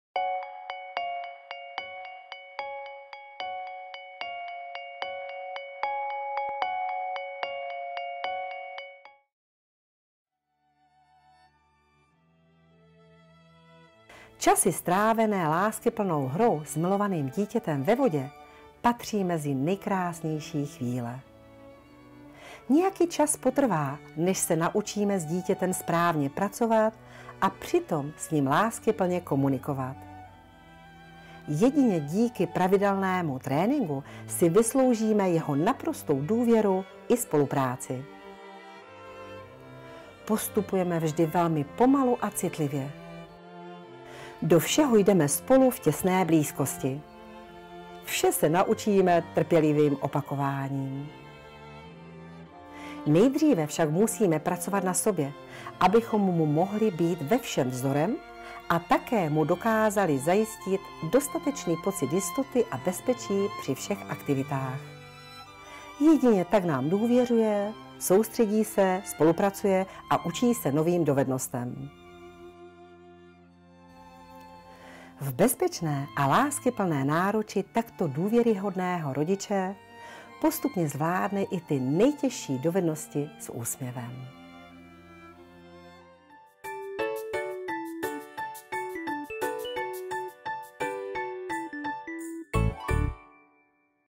Jak se dá plavat s půlročním dítětem?
Otázky rodičů a odpovědi Evy Kiedroňové

Tentokrát na téma: Jaký má význam plavání s miminky? Jak s nimi plavat, aby je to bavilo a rozvíjelo?